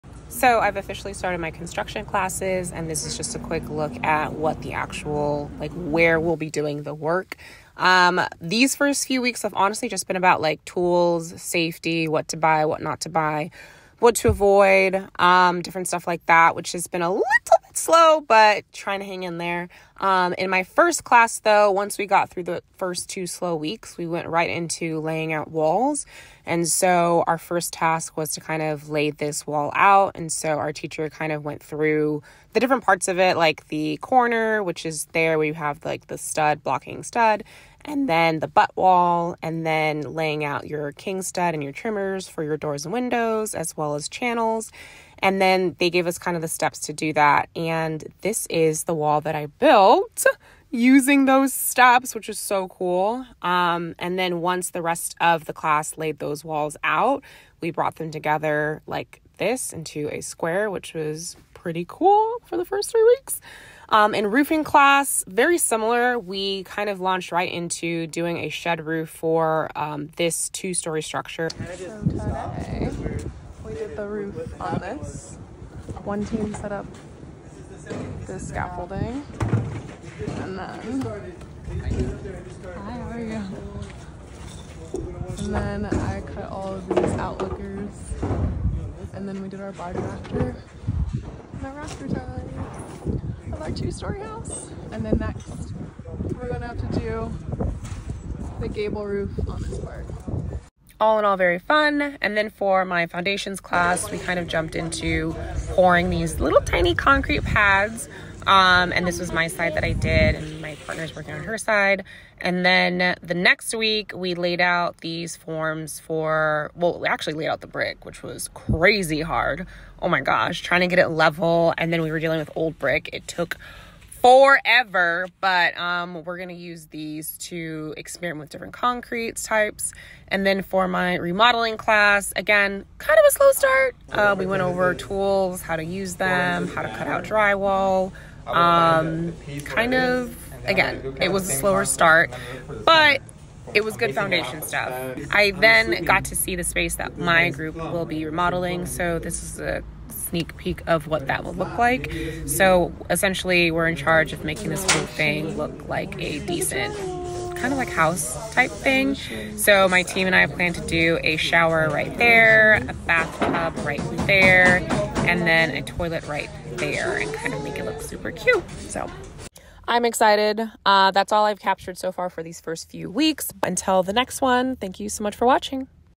Ep. 003 | Becoming a Developer | School Begins
School is underway and I’m set to learn about:
- Foundations
- Framing
- Roofing
- Interior Remodeling

My framing class kicked into full gear with us started out by laying out and building a wall.

Home remodeling, however, has been a different story so far.

➡️➡️ LA City Pre-Approved ADU Plan

➡️➡️How to Become a Real Estate Developer - The Workbook (Print) 📚📋:
Fulfilled by Amazon